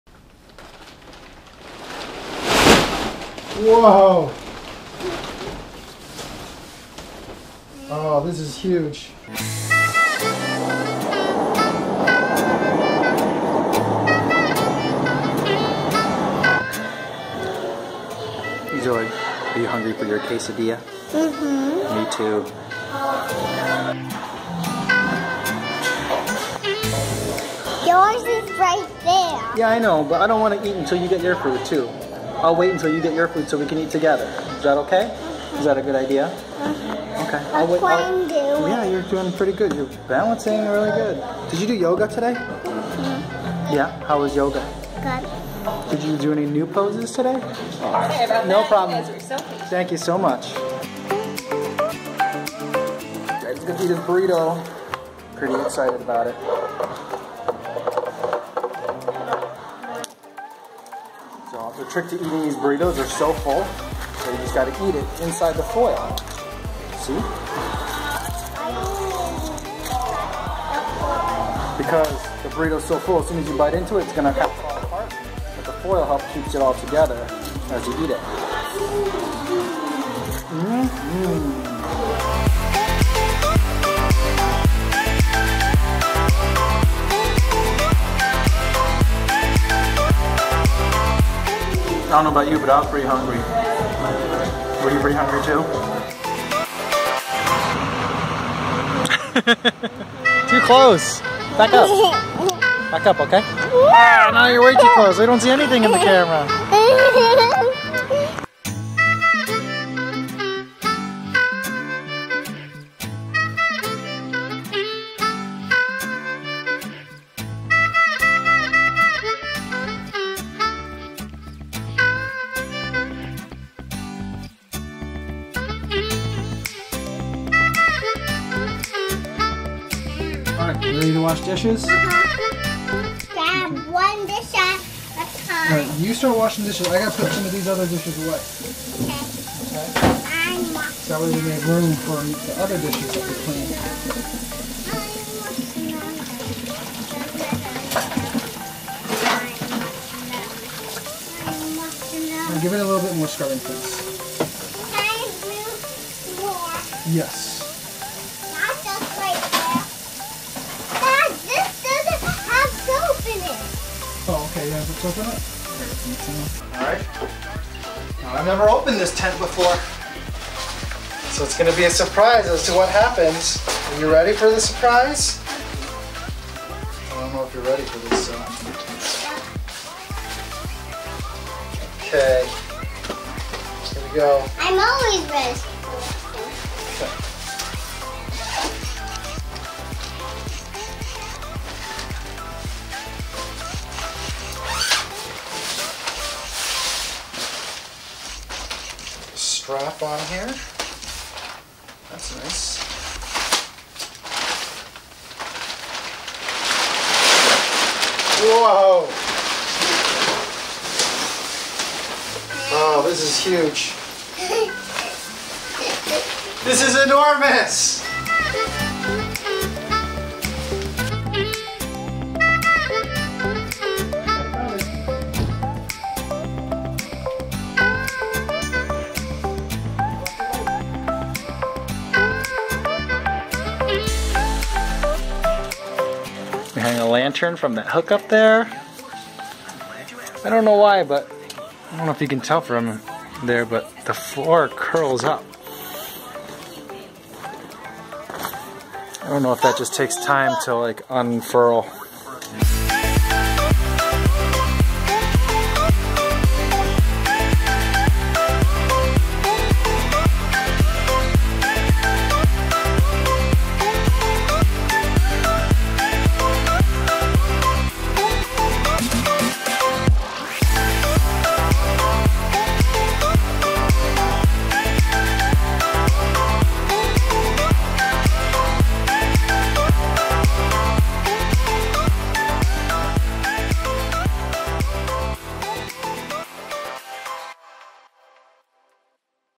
GAZELLE INSTANT POPUP CAMPING TENT | vlog 128
It was another Chipotle Monday for me and my daughter. My wife left town for work this week, so it was just the two of us. After dinner, we headed home to take care of some errands and make some crafts before bedtime.

Whenever my wife leaves town for work, I try to take my daughter's mind off of things by promising that we can camp in the living room. We had been sleeping under our popup sunshade. But since I still hadn't had a chance to use the Gazelle Instant Pop up Camping Tent, I thought this would be the perfect opportunity.

beme: kofuzi

shot on:
nikon d3200

gopro hero 3+ black (certified refurbished)

moto g 2015